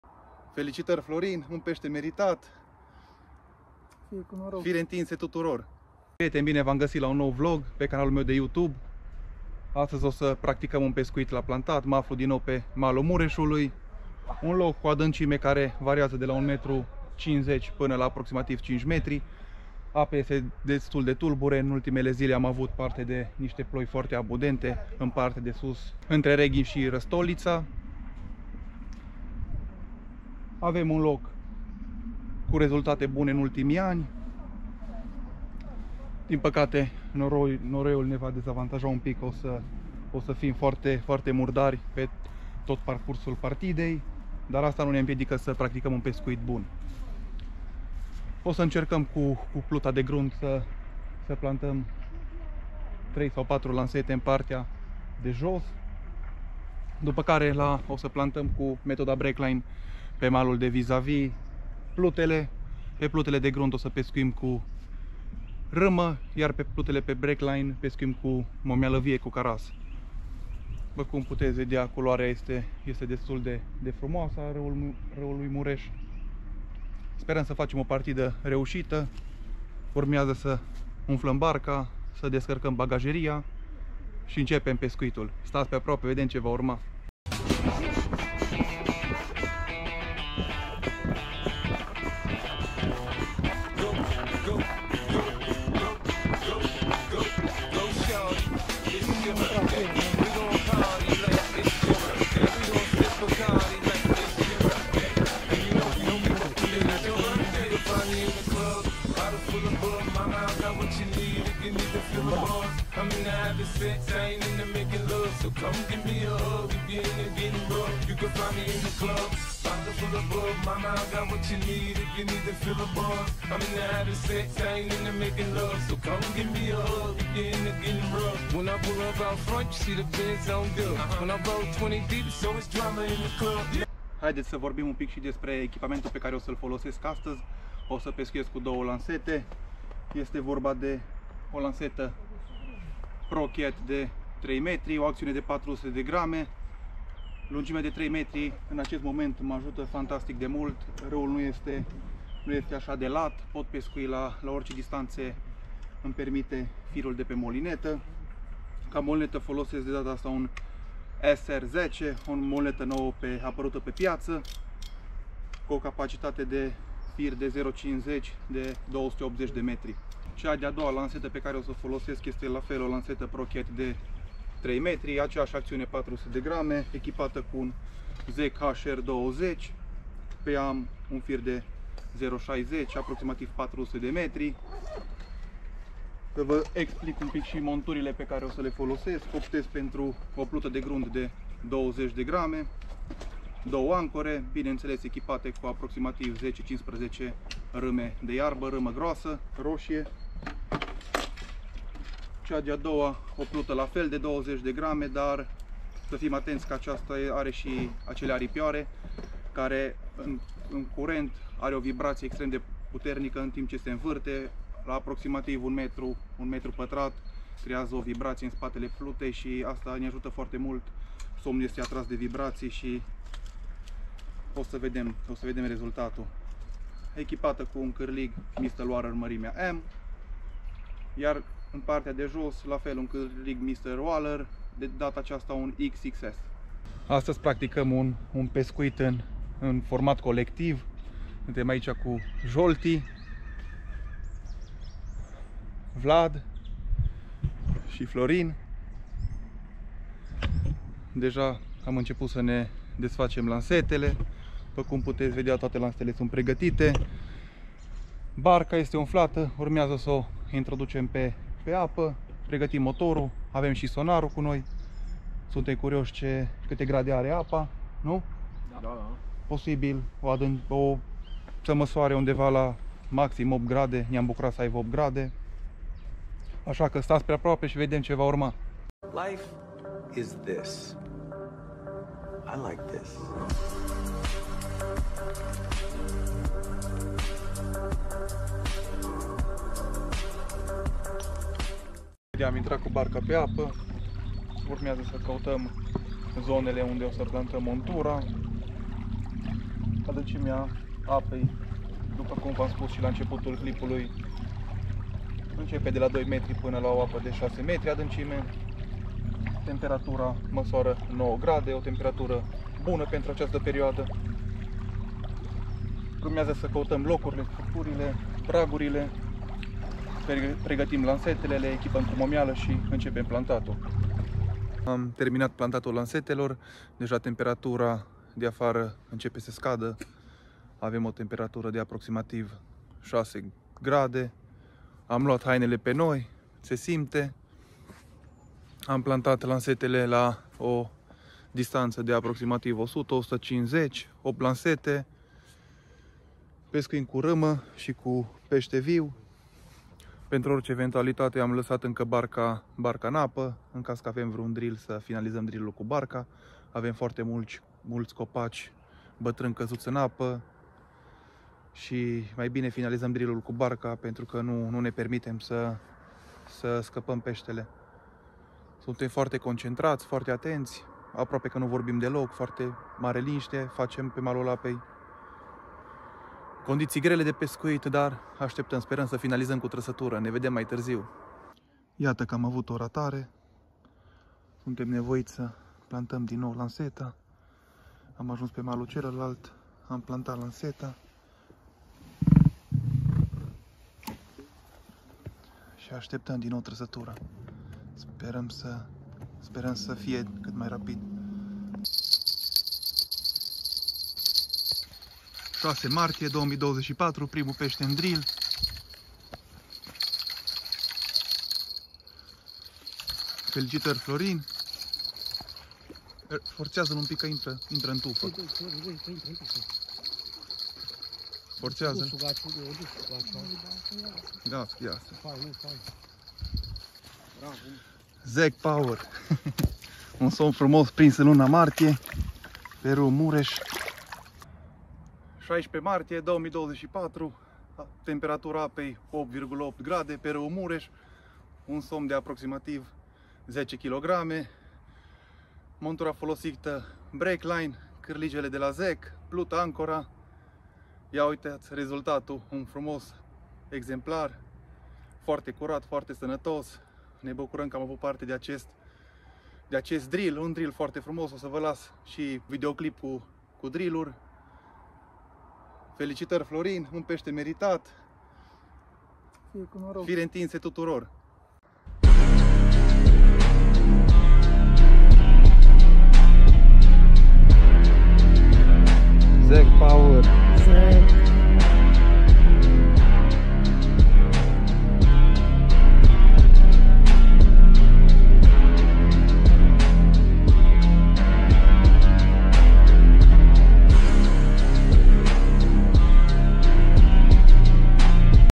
Raul Mures.Pescuit la somn plantat in sezon rece.8,8 grade
In data de 16 Martie am hotarat impreuna cu prietenii mei Florin Candea , Jolti Reti si Truta Vlad sa organizam o iesire la pescuit plantat pe Raul Mures.Stiind ca conditile meteo sunt nefavorabile reusim sa capturam un evemplar de somn masurand 1 metru lungime.
produsele fololsite de mine pentru pescuitul plantat la somn se numesc Zeck Fishing si le gasiti la magazinele partenere @mimteam7953